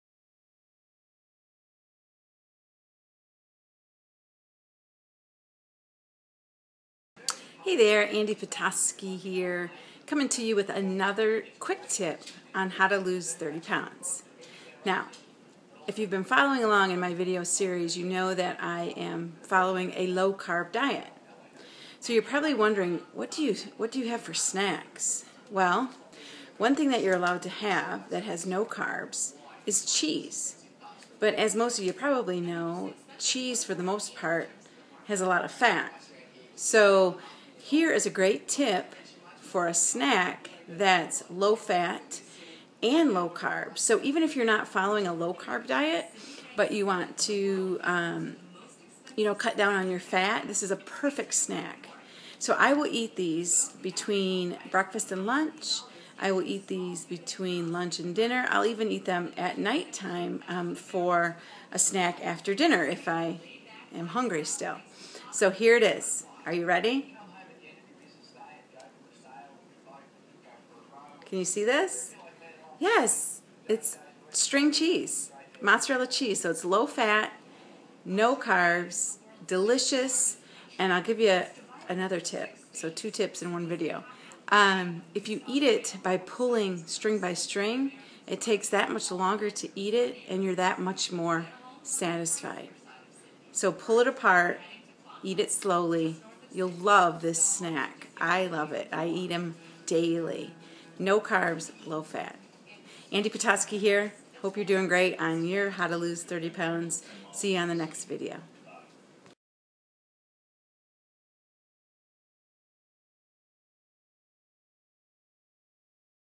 How to Lose 30 Pounds - Quick Tip #4
So, here it is... Quick Tip #4! WOOT! WOOT!

You are not going to believe what snack I am talking about in this video...

And, guess what? This is a GREAT snack even if you are not on a low carb regimen!

Love, love, love this snack!